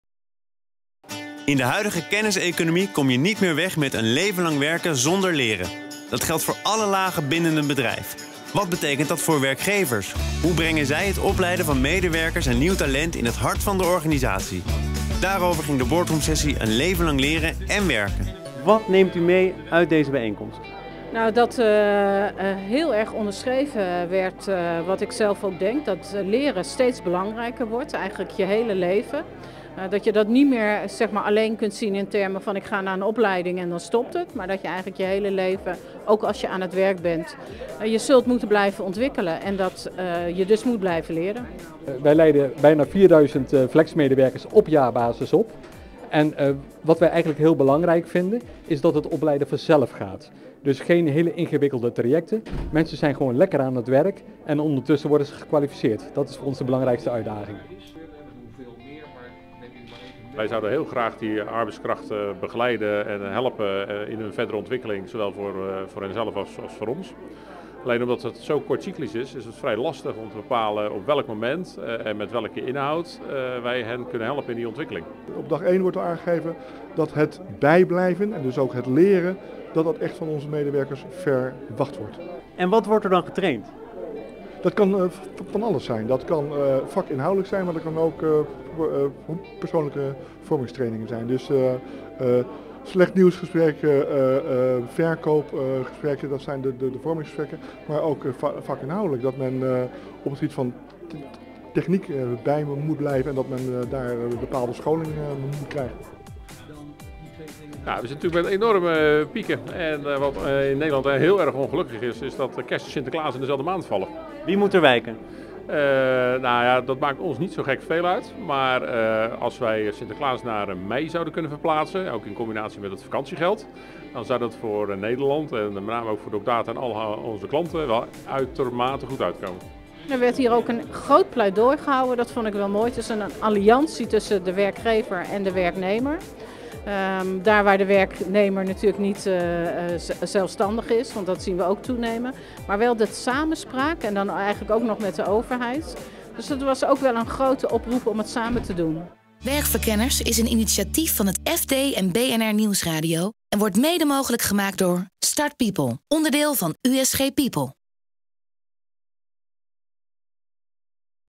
Werkverkenners | Boardroomsessie Een leven lang leren
Een leven lang leren staat hoog op de agenda van de politiek, het bedrijfsleven en de vakbonden. Maar op welke wijze breng je dit in de praktijk? Daarover ging de derde Werkverkenners boardroomsessie op dinsdag 24 november.